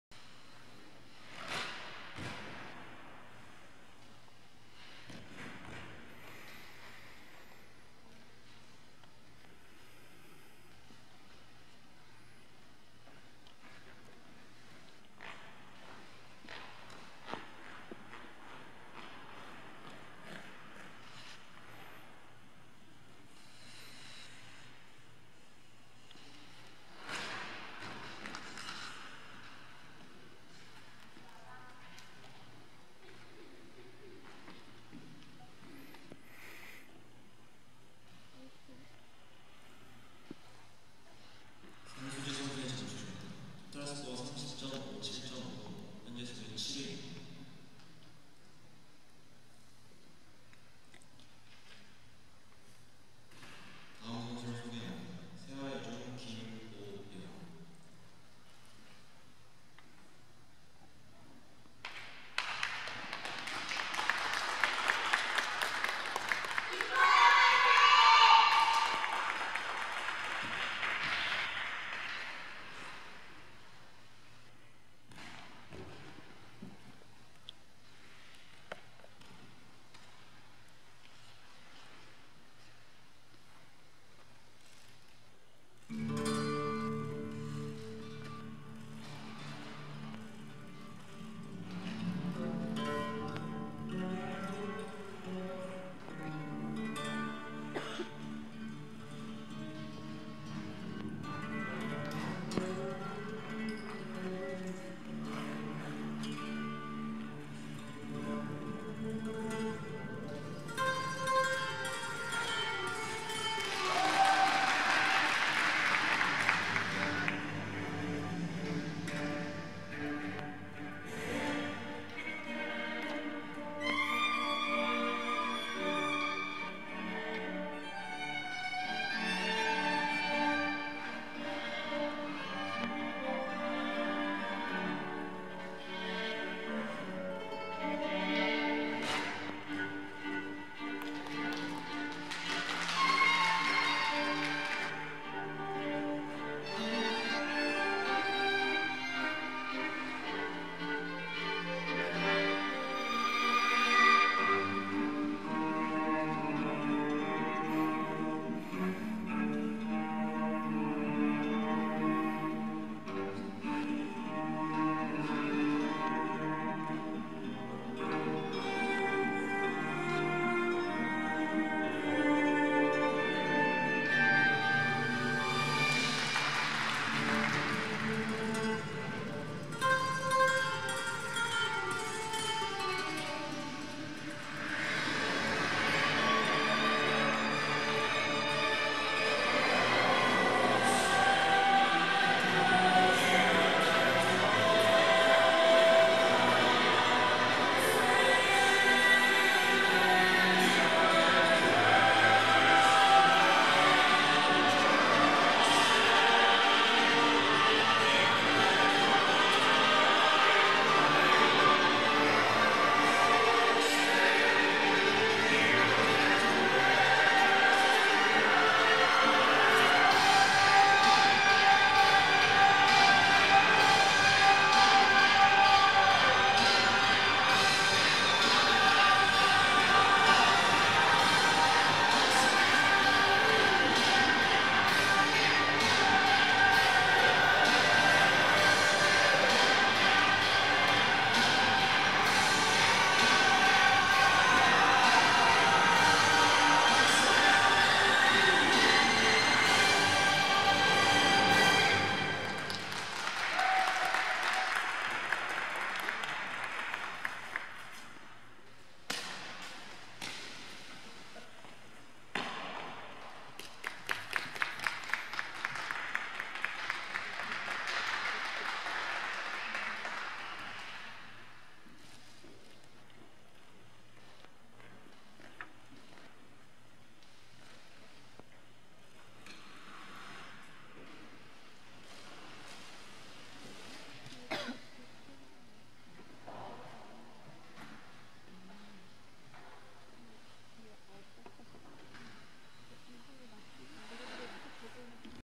김보영 Bo-Young KIM | SP | 2016-04-11 종별선수권 | 여자중학부 싱글B조 11 | 세화여자중학교 2학년
캡쳐 및 재가공 금지 |
음원 및 음원으로 인한 영상 문제시 다음팟 플레이리스트 | 구글측 삭제임 |

제58회 전국남녀 피겨스케이팅 종별선수권대회 | (08-12.Mar.) 2016. Day4. |
여중부 B조 B Ladies(Junior) Middle School | Short Program
인천선학국제빙상장 | Seonhak International Ice Rink